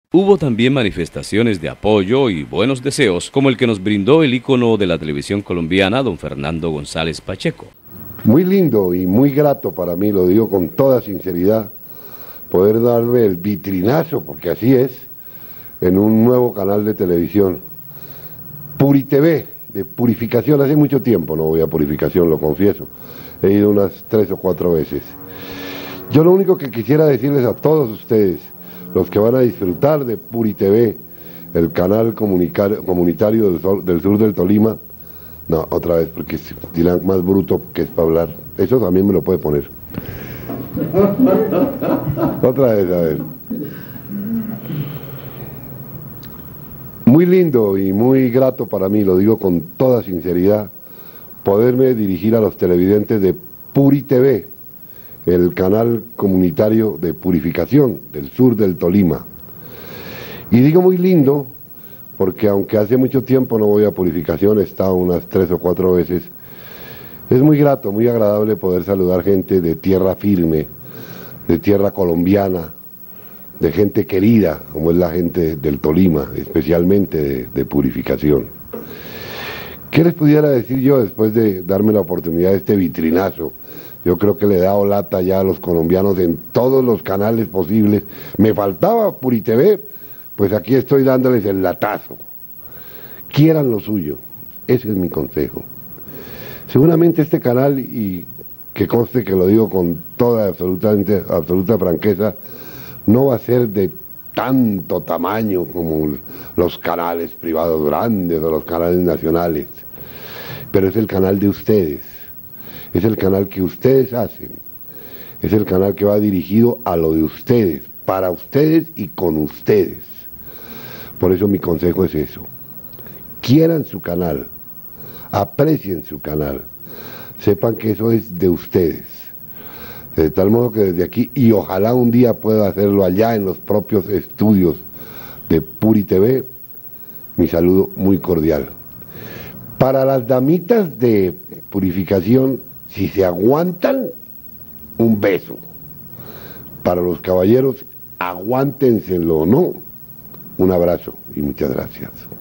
Fernando González Pacheco Canal Puri Tv
En el Año 2000 Fernando González Pacheco nos concedió esta entrevista la cual le daba la bienvenida  y le deseaba buenas cosas al Canal Puri Tv y nos dio varios consejos para la realización de la televisión comunitaria...